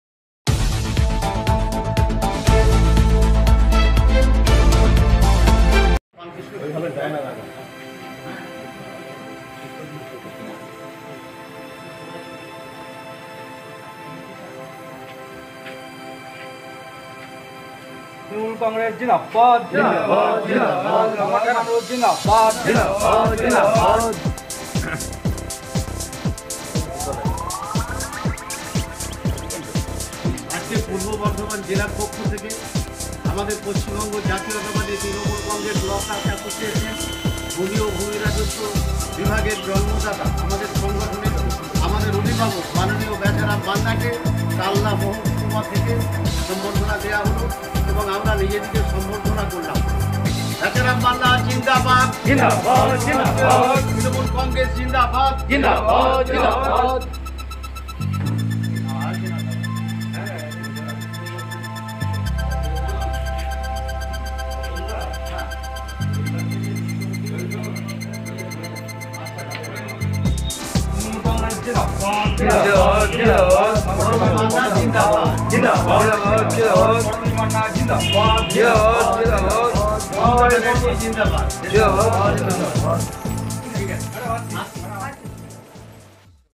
রাজ্যের শ্রমমন্ত্রী বেচারাম মান্না ও বিধায়ক করবী মান্নাকে সম্বর্ধনা tmc ল'ক্লার্ক এসোসিয়েশনের।
রাজ্যের শ্রমমন্ত্রী বেচারাম মান্না ও বিধায়ক করবী মান্নাকে সম্বর্ধনা দিলো তৃণমূল কংগ্রেস ল'ক্লার্ক এসোসিয়েশনের কালনা ইউনিট।